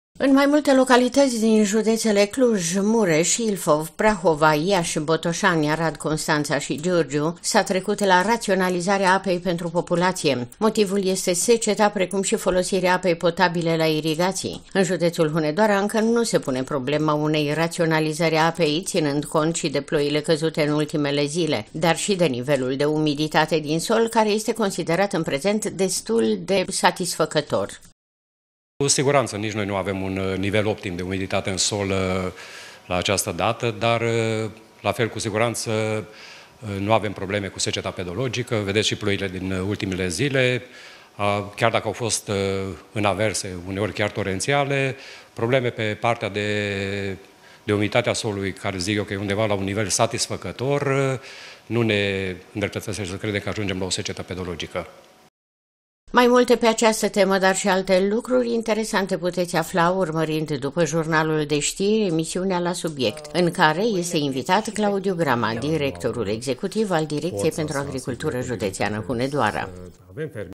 JUDEȚUL HUNEDOARA NU ESTE, ÎNCĂ, AFECTAT DE SECETĂ
Dacă în mai multe județe din țară se pune problema raționalizării apei, atât pentru consumul populației, cât și pentru agricultură, în Hunedoara încă nu se pune problema. Specialiștii din domeniu afirmă că, la această dată, nivelul de umiditate întâlnit în sol este unul satisfăcător.